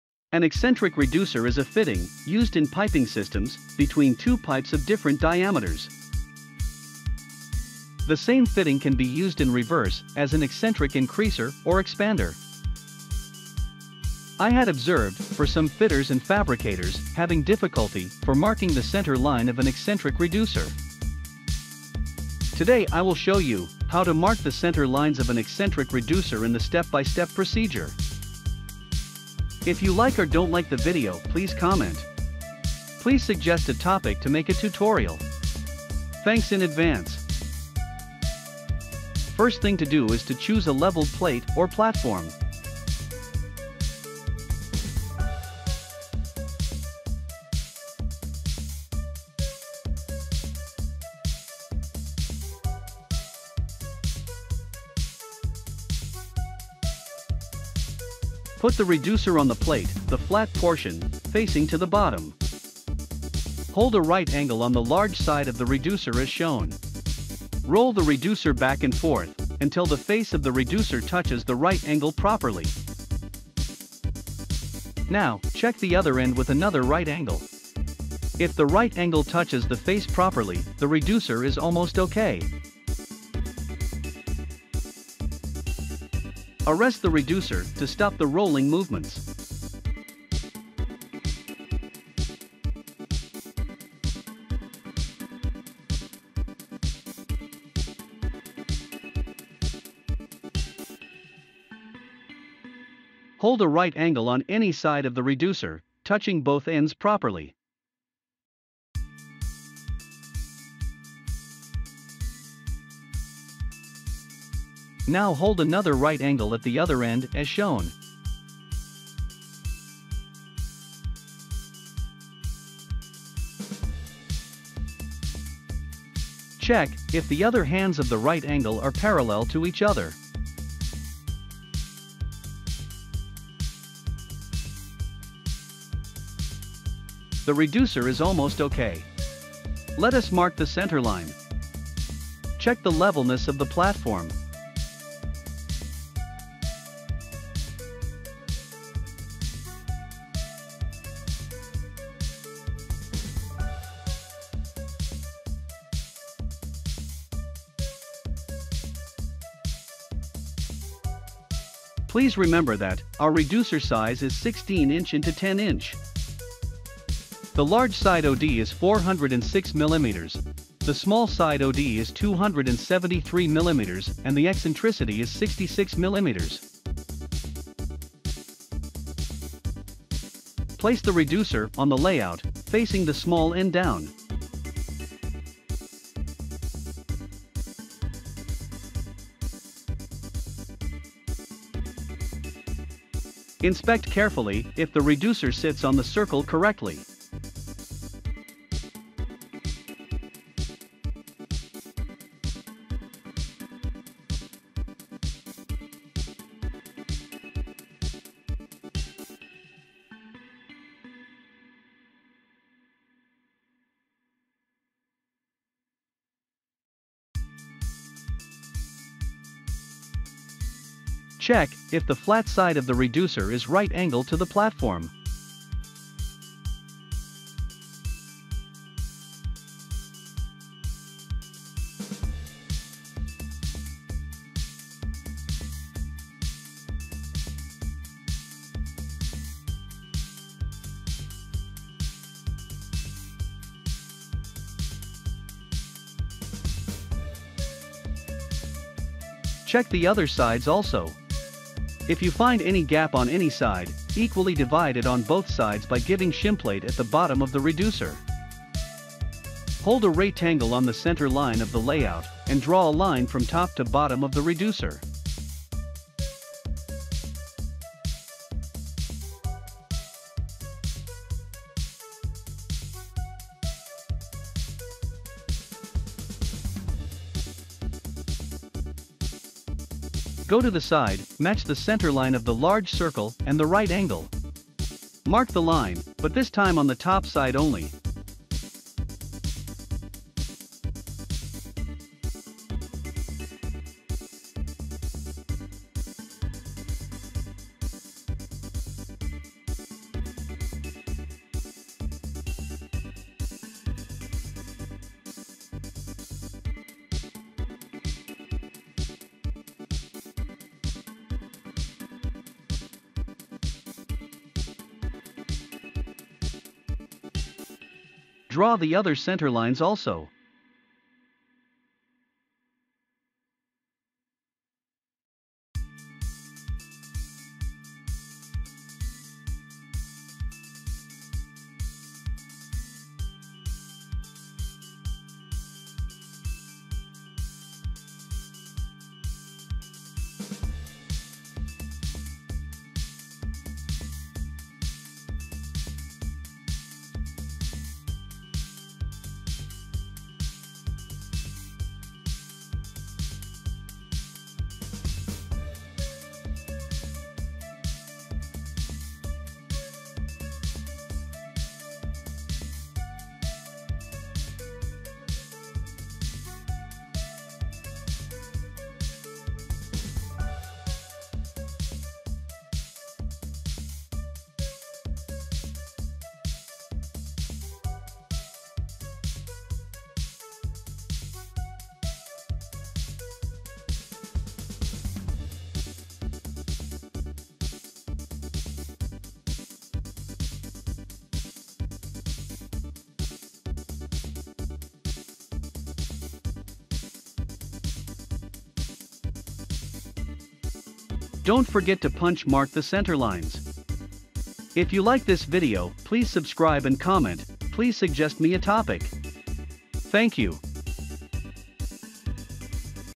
How to mark the center line of an eccentric reducer. Step by step method for beginners. explained.
Simple and step by step procedure for marking the center line of an eccentric reducer.
FOR PDF FILES
1 Elbow Radius Chart